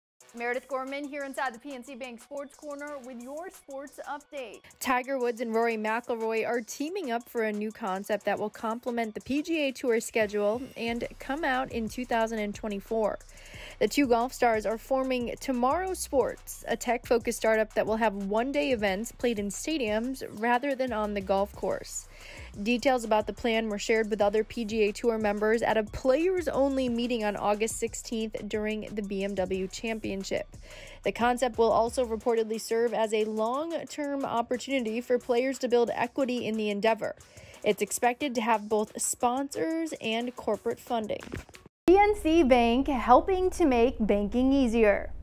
Tiger Woods, Rory McIlroy Teaming Up With PGA Tour To Launch New Golf Concept
Tiger Woods and Rory McIlroy have teamed up with the PGA Tour to launch a new golf concept that may involve playing golf in stadiums.